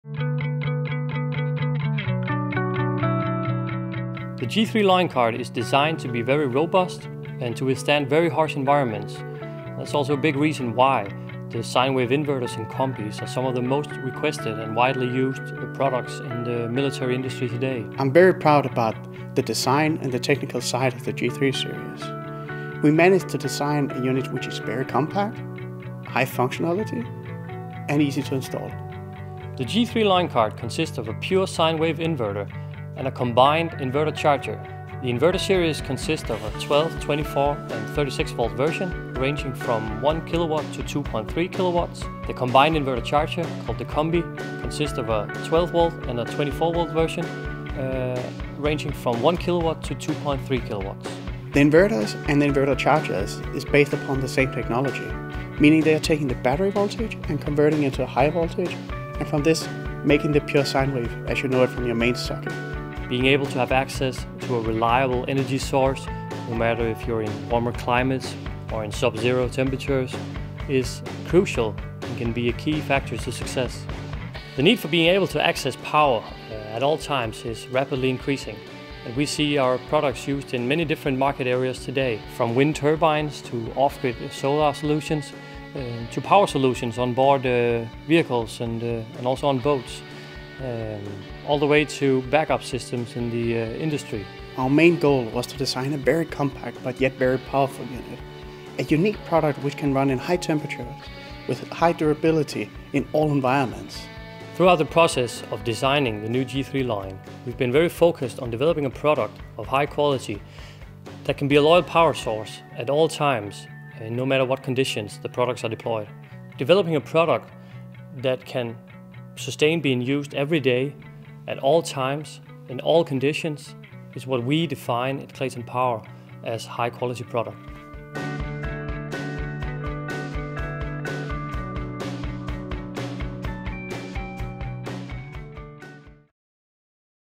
G3 Sine Wave Inverter and Inverter/Charger presentation by Clayton Power
The G3 sine wave inverter and inverter/charger series are very compact and made for use in harsh environment. The sine wave power inverters and inverter/chargers gives opportunity to use mobile 230 volt power anywhere, anytime. All the DC/AC products (sine wave inverter) and inverter/charger) have a pure sine wave output curve. For further information about the sine wave inverter power systems, and for information about the inverter/chargers